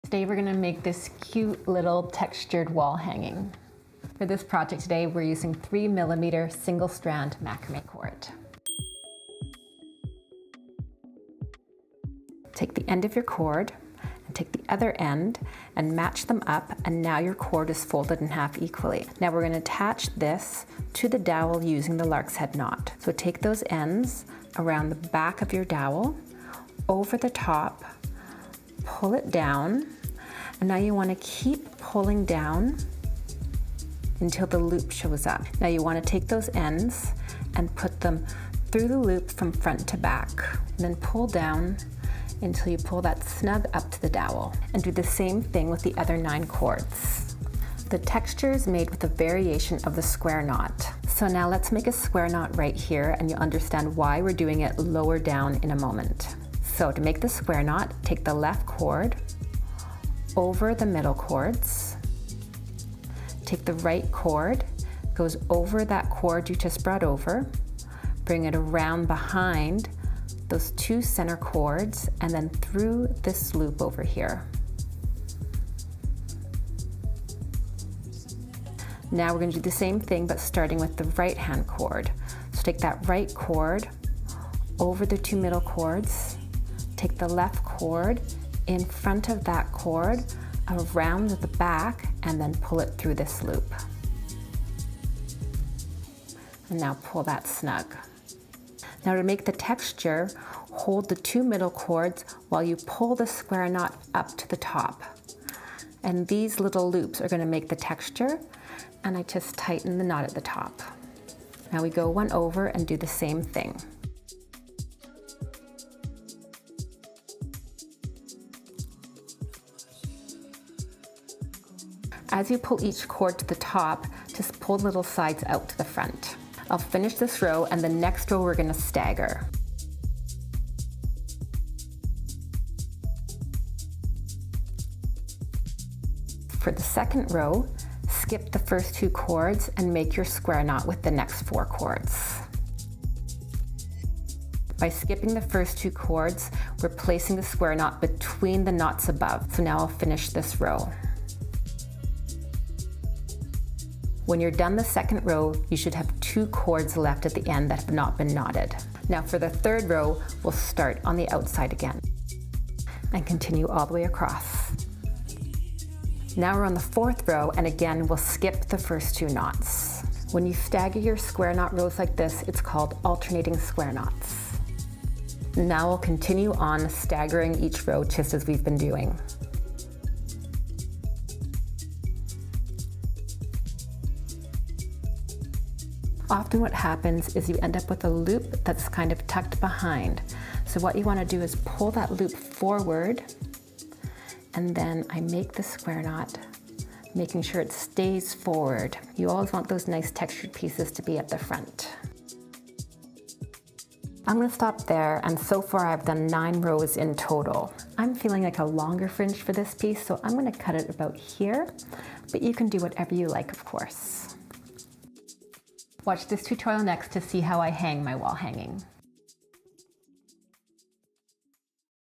HOW TO make a mini textured macrame wall hanging! Perfect for the absolute beginner!!!
Super easy and quick mini macrame wall hanging tutorial.  This project is perfect for the beginner to macrame.  In this tutorial you will learn how to make the lark's head knot and the square knot (which is the knot used to make the texture).  Follow along with this step-by-step tutorial and you'll be delighted with the result!

MATERIALS:

Dowel - approximately 18cm (7")
10 lengths at 2 meters (40") of  3mm single strand macrame cord.

Here is a link to the MB Cordas 3mm cord I use.

CHAPTERS:
0:00 - Intro
0:09 - Cut lengths & dowel size
0:13 - Lark's Head Knot tutorial
0:48 - Square Knot Tutorial & How To Make the Texture
3:28 - Cutting the Fringe